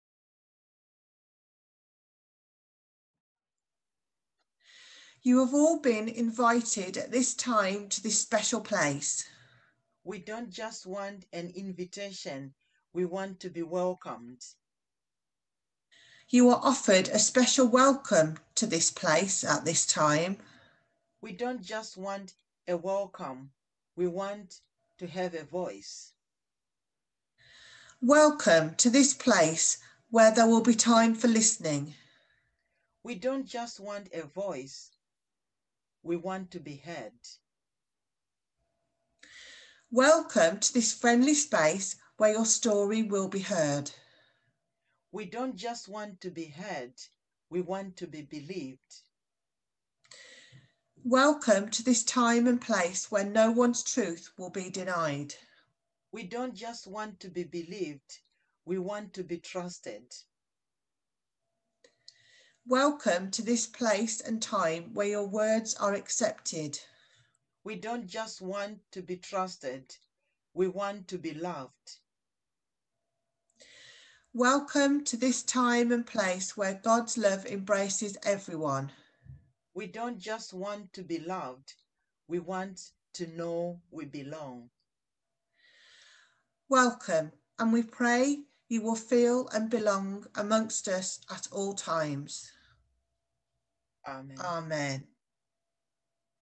Worship to mark Racial Justice Sunday: "A True Welcome"
A reflection on what a "welcome" truly entails, led by Rev'd Molly Chitokwindo and Liz Dean, EDI advisers in the Wolverhampton & Shrewsbury District.  Adapted from a reflection by Richard Becher.
Part of a suite of worship resources prepared in the District for Racial Justice Sunday 2021